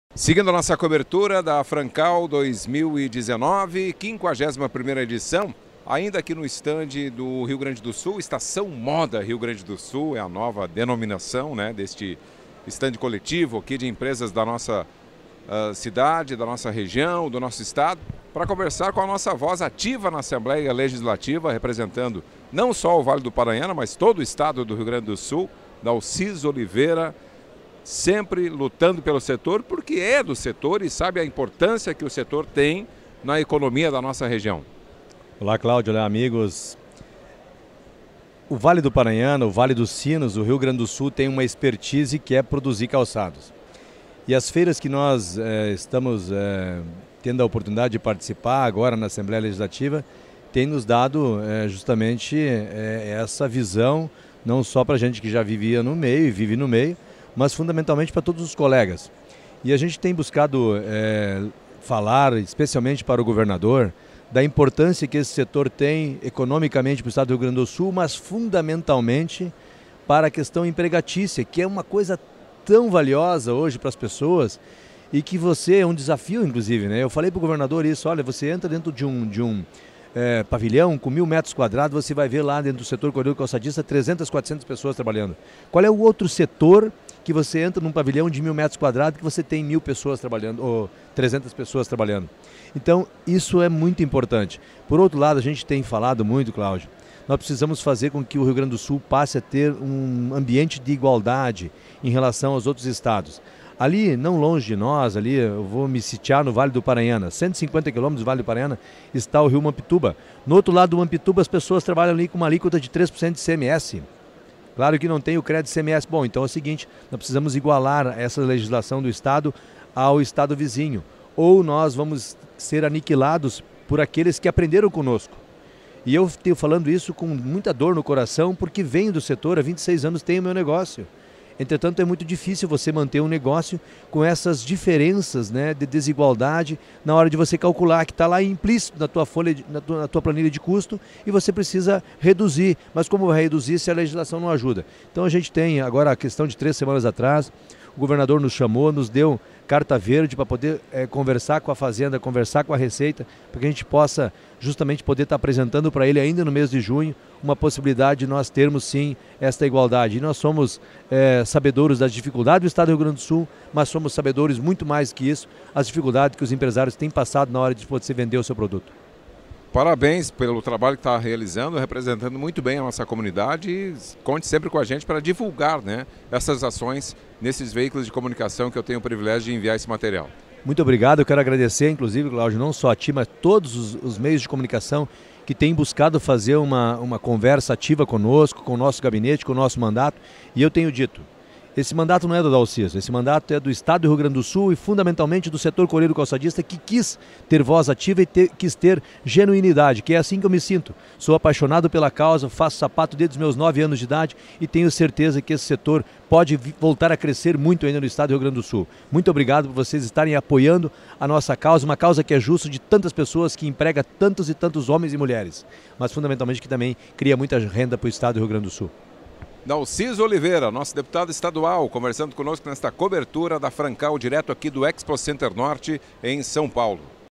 FRANCAL 2019 - Dalciso Oliveira
Assista uma entrevista com Dalciso Oliveira Deputado Estadual no RS na cobertura da 51ª FRANCAL direto do Expo Center Norte em São Paulo.